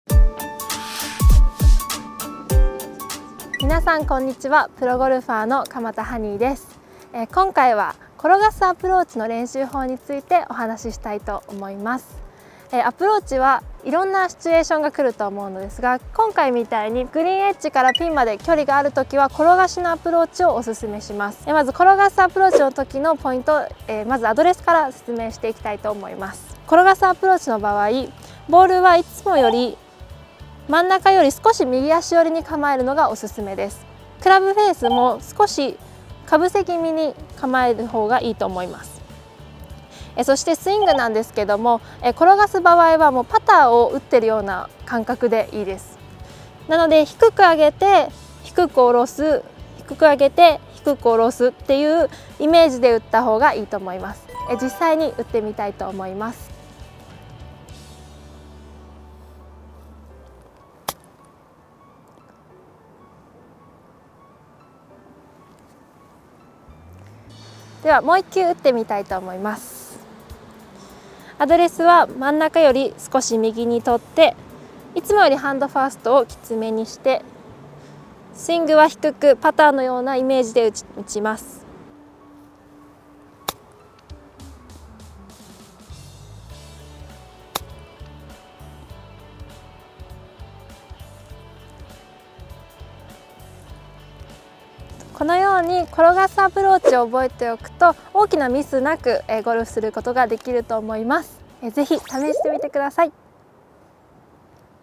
転がしアプローチの練習で注意すべきポイントとは？【ゴルファボ】
◆ゴルファボLINE公式アカウント
友達登録はこちら↓ 無料で上達法が届きます。

転がしアプローチでは重要なことは、ボールを上げすぎないようにしっかりとロフトを寝かしたまま当てることです。

ボール位置や握り方などを極度に変えてしまうとダフリやトップの可能性が高くなり、思ったように転がすことができません。

プロがおすすめするミスショットしないための転がしアプローチのコツをご紹介します。